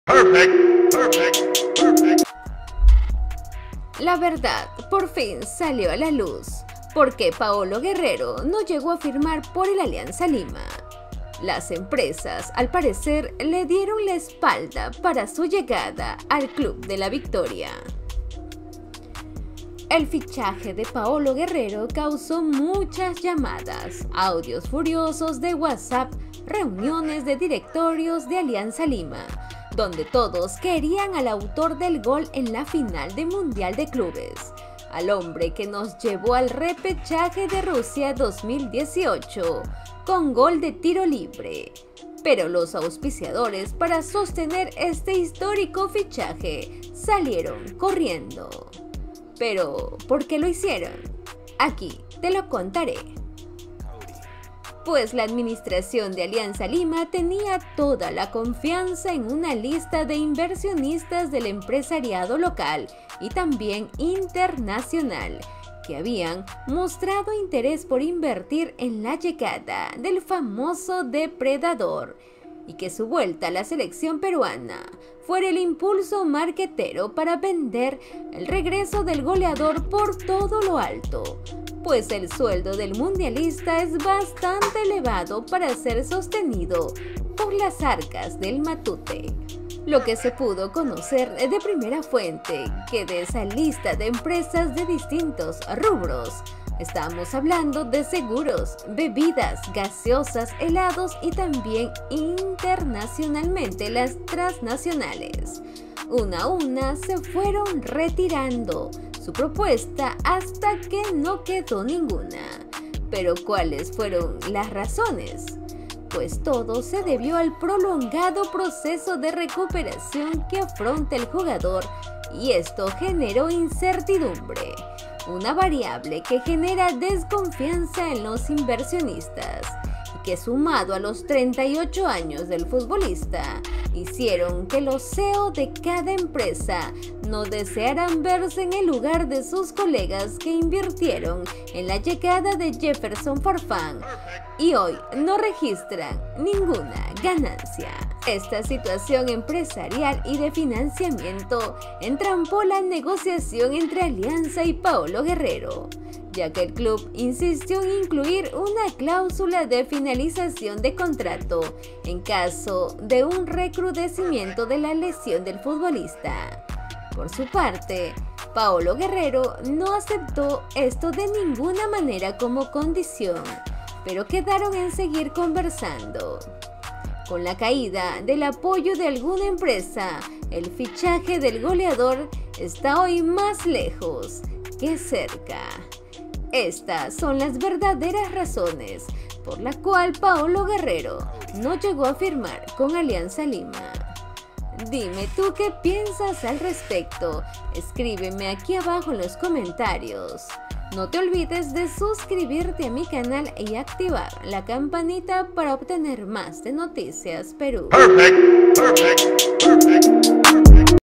MARC4S CHOTEAN A PAOLO GUERRERO! EMPRESAS LE DAN LA ESPALDA PARA SU LLEGADA AL AL
Inversionistas nacionales e internacionales le dan la espalda a Paolo Guerrero por el alto precio que pide como sueldo aun como lesionado, las empresas no se quieren arriesgar como lo hicieron con Jefferson Farfan y le dicen no a Guerrero.